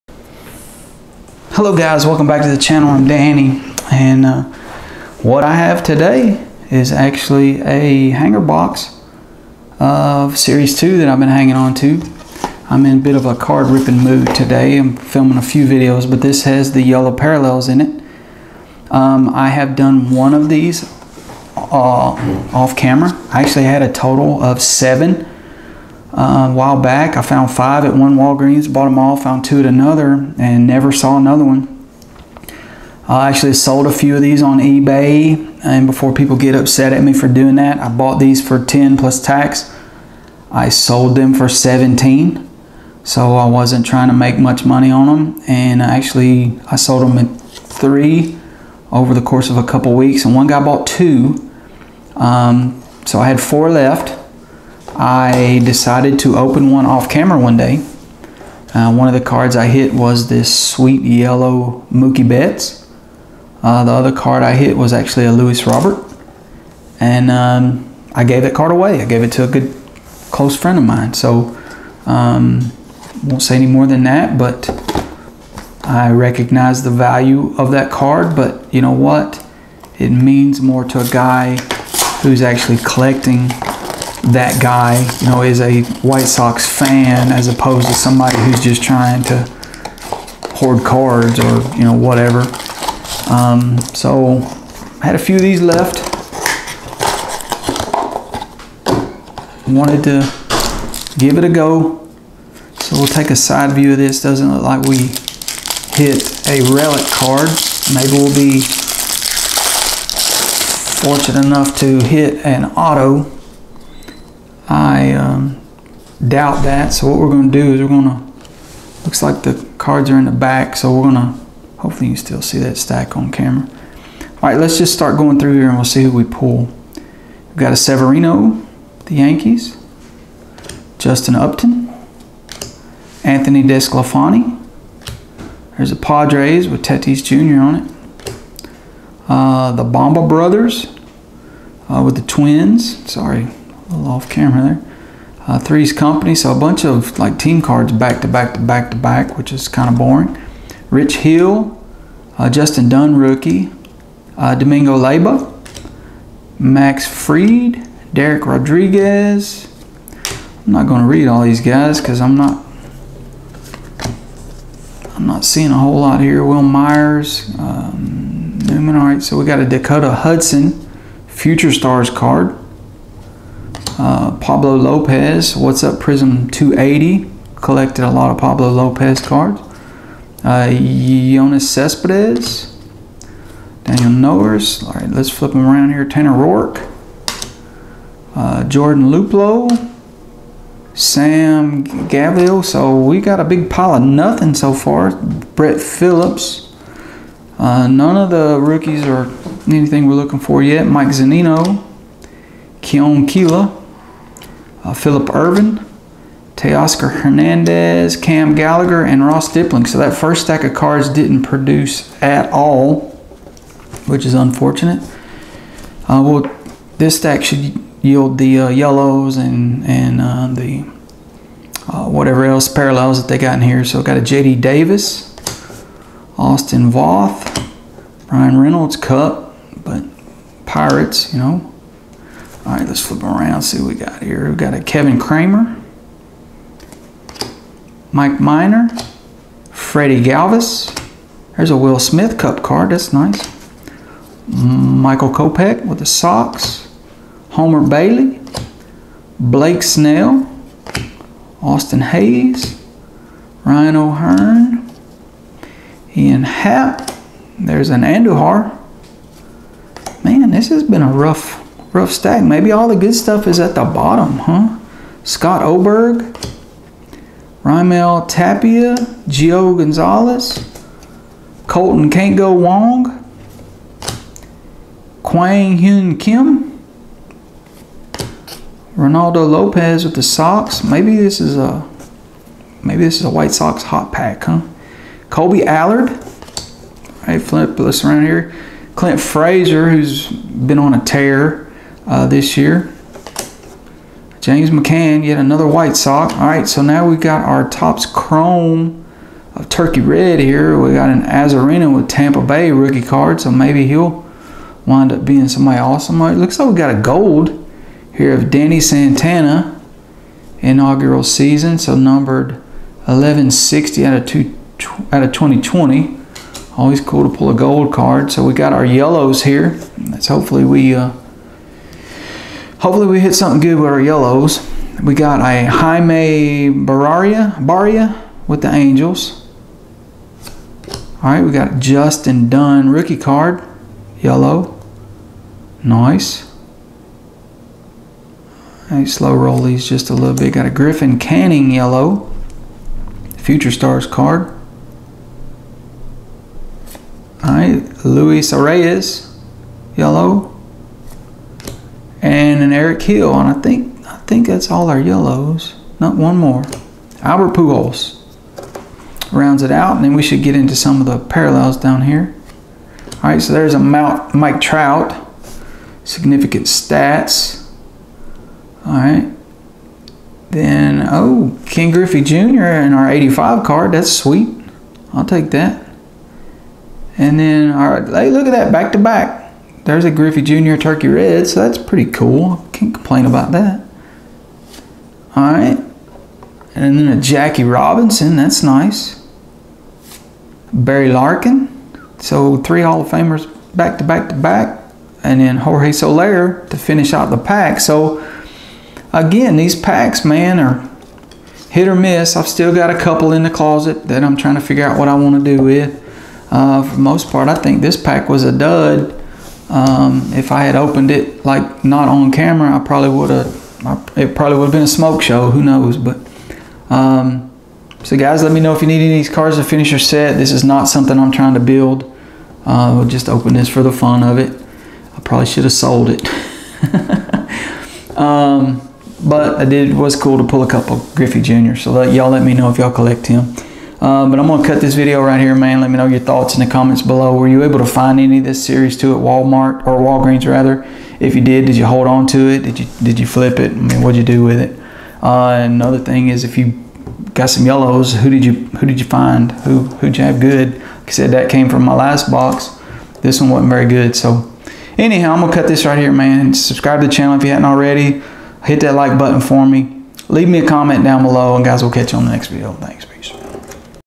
Ripping a Topps Series 2 Walgreens Hanger Box - Yellows!
For this video, I rip a hanger box of series 2 that I have had in the closet for a couple months.  This wasn't as epic a rip as I had hoped, but it is cool to be ripping product again.  Let me know your thoughts on the yellow parallels.  They are some of my favorite cards out there.